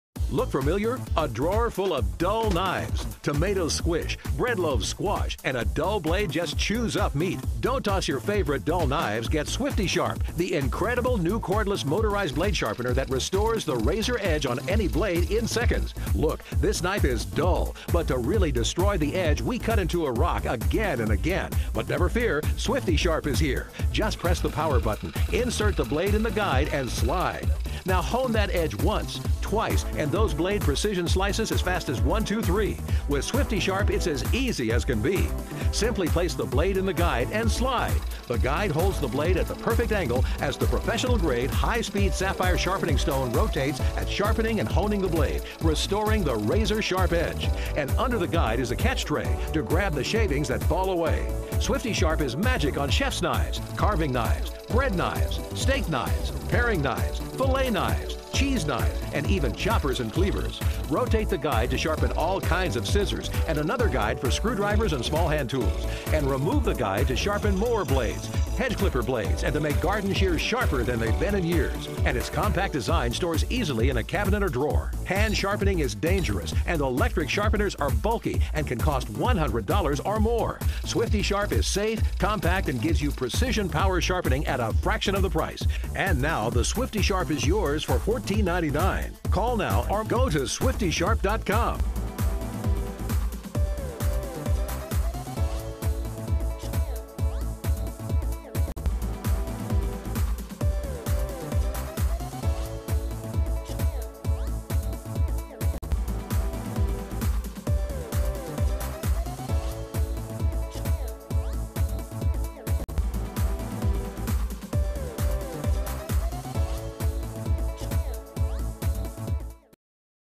How to sharpen blunt knifes effectively
Get a second life out of your knives, scissors and other household products with the Swifty Sharp cordless multipurpose blade sharpener. Equipped with a professional grade, high-speed sharpening stone, the Swifty Sharp restores the razor's edge on any blade in just seconds, from knives to scissors to small hand tools.

Features:

Cordless and battery operated
Professional grade, high-speed rotating sapphire stone shapes and hones the blade
Compact design for easy storage
Safe and easy to use